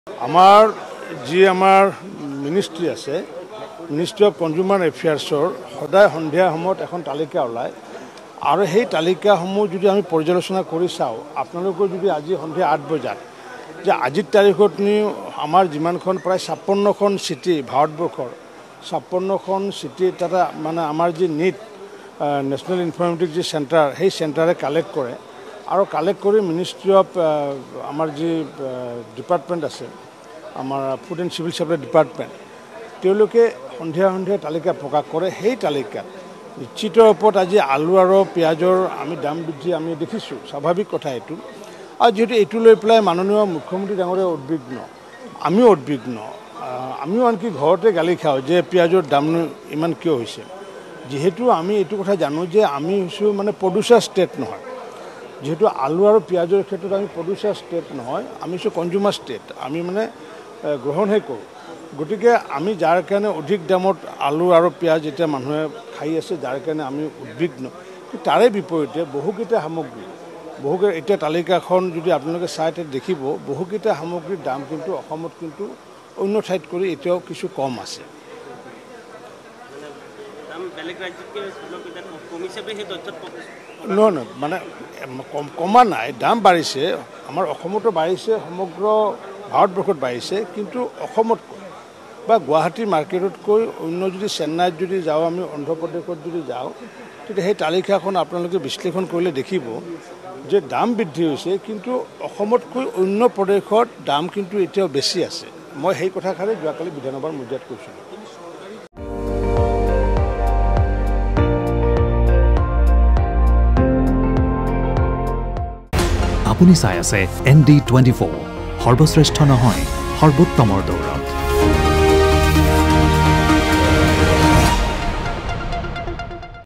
Ranjeet Kumar Dass On Price Hike: সামগ্ৰীৰ দাম বৃদ্ধি সন্দৰ্ভত ৰঞ্জিত দাসৰ মন্তব্য
'দেশৰ অন্য ৰাজ্যসমূহৰ তুলনাত অসমত দাম কম'৷ সামগ্ৰীৰ দাম বৃদ্ধি সন্দৰ্ভত ৰঞ্জিত দাসৰ মন্তব্য...